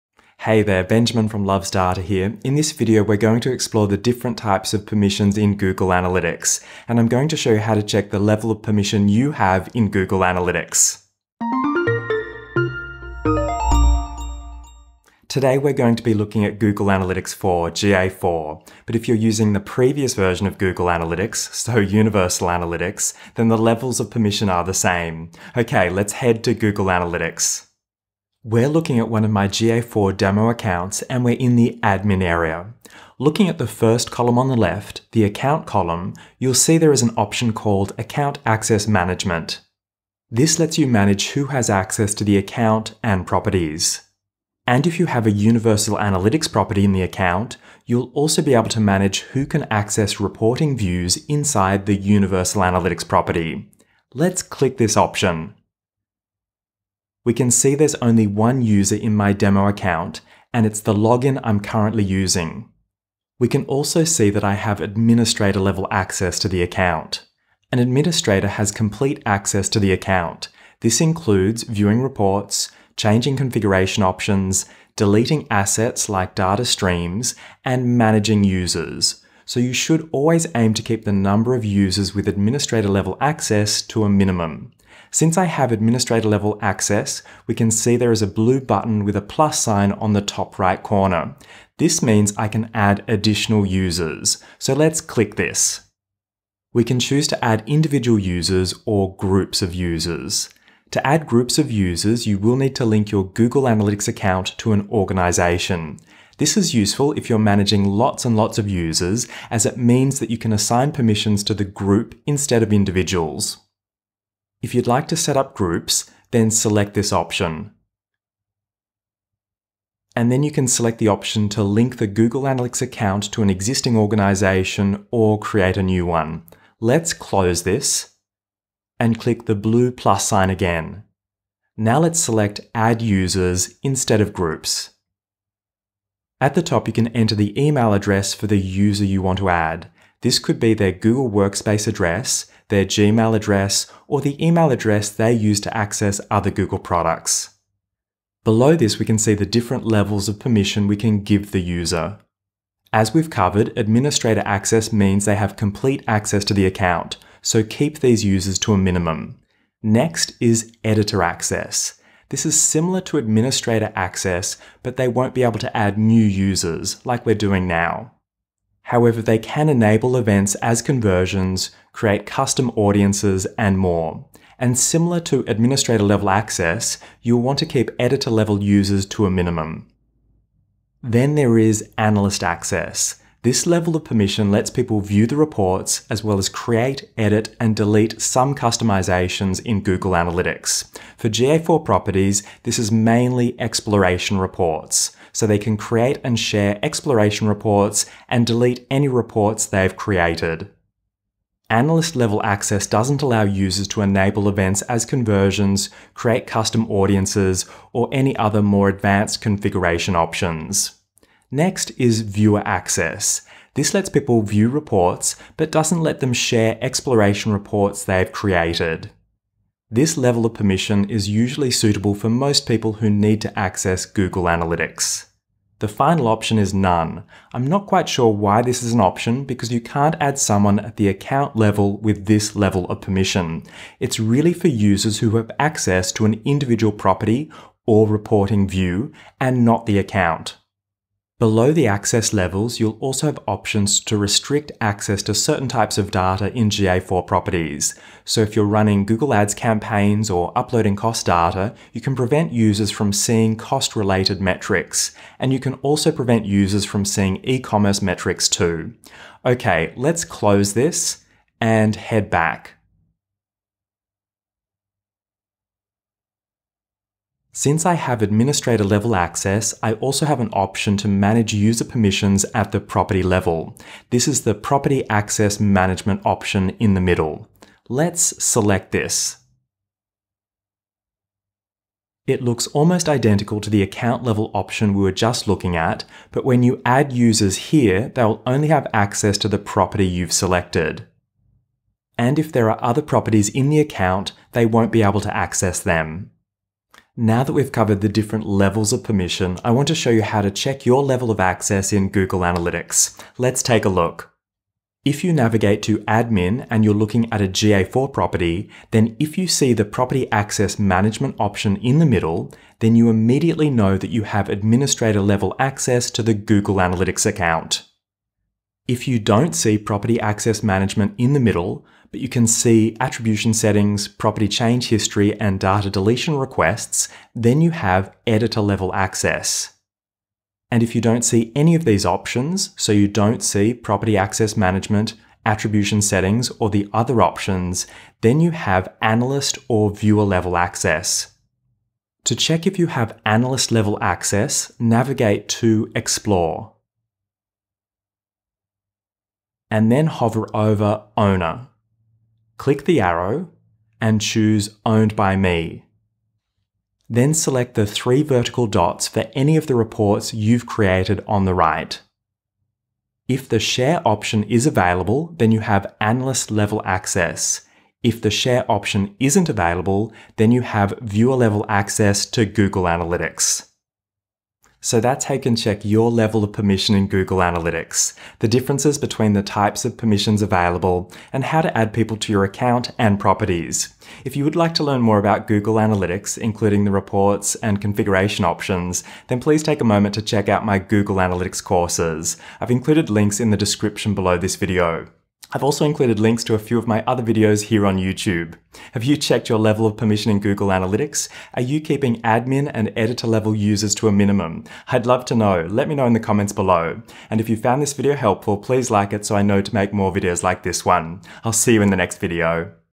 Google Analytics User Permissions – Administrative, Editor, Analyst & Viewer Permissions
Learn about the different types of user permissions in Google Analytics, including administrative, editor, analyst, and viewer permissions. You will also learn how to check your own level of permission in Google Analytics.

JUMP TO A TOPIC IN THIS VIDEO:
0:00 Introduction
0:27 Account access management
2:15 Google Analytics user permissions
4:27 Property access management
4:56 How to check your permission
6:14 Conclusion

My book

SAY HELLO!